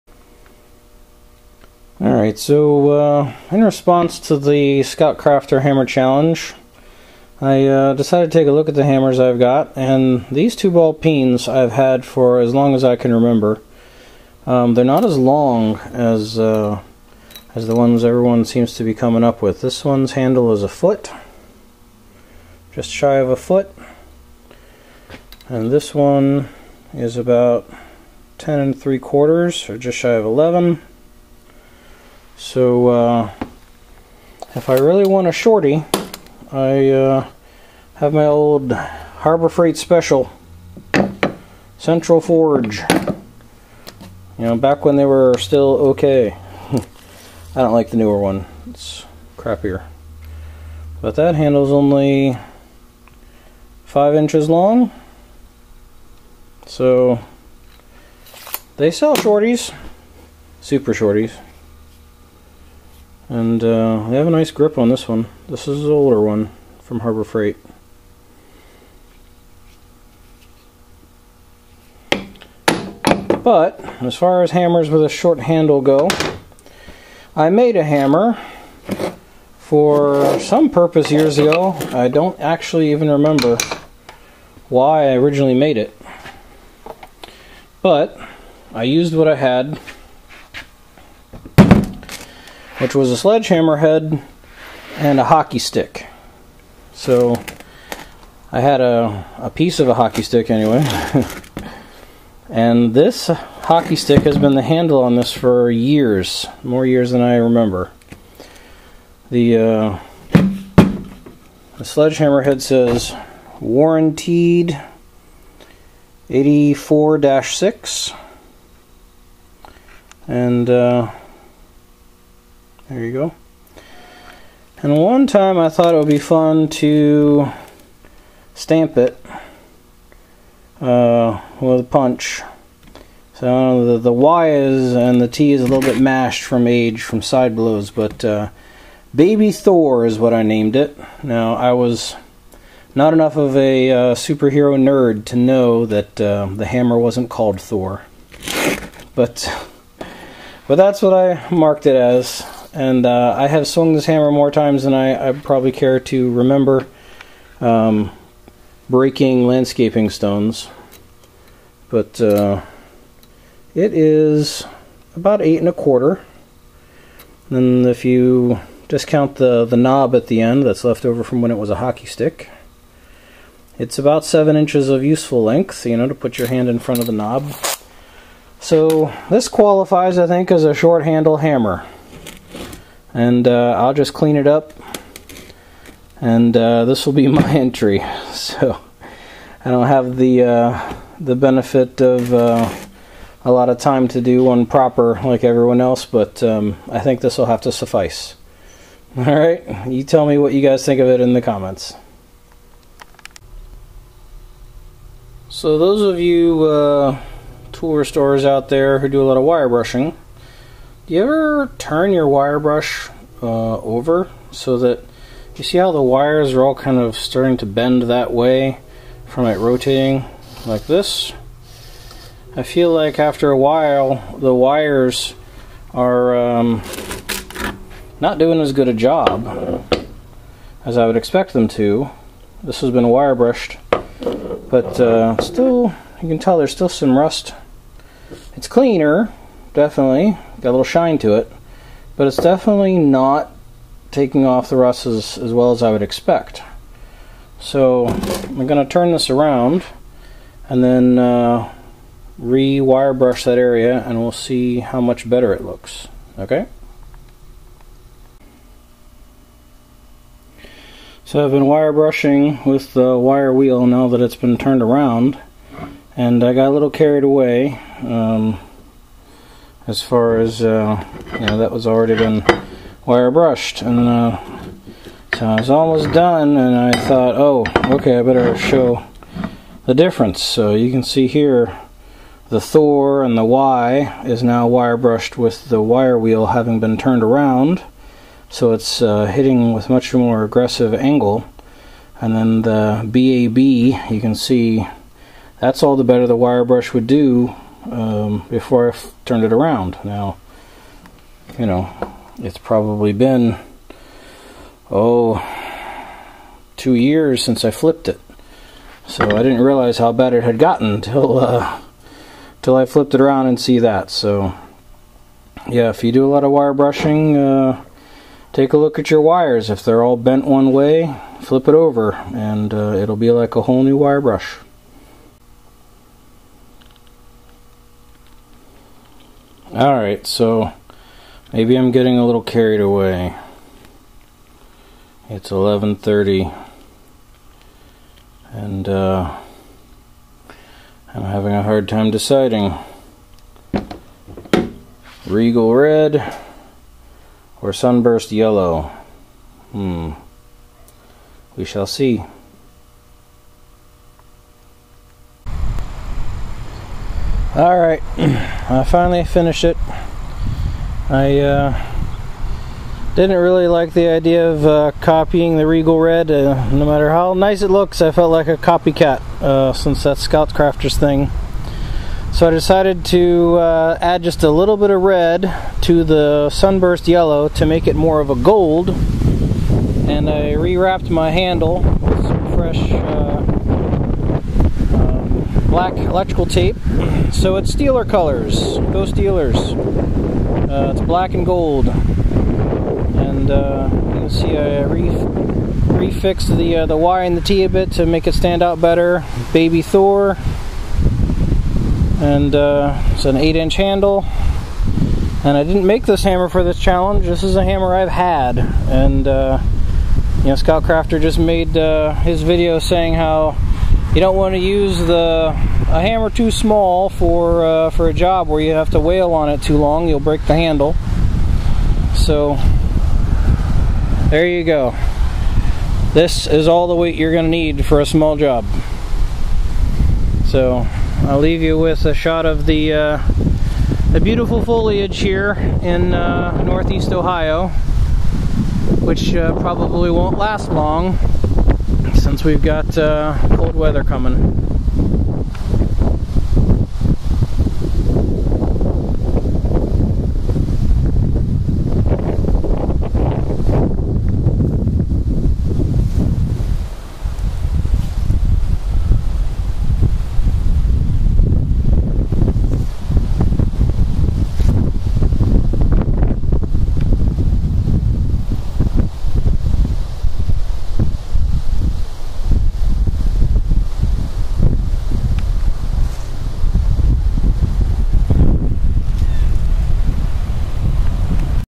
Scoutcrafter Short Handled Hammer Challenge
My entry for the ScoutCrafter Short Handled Hammer Challenge and a shop tip in the middle.  Sorry about the wind noise at the end, but after I show the finished hammer, it's all "extra credit" as Mr.Pete would say.
Tool Restoration.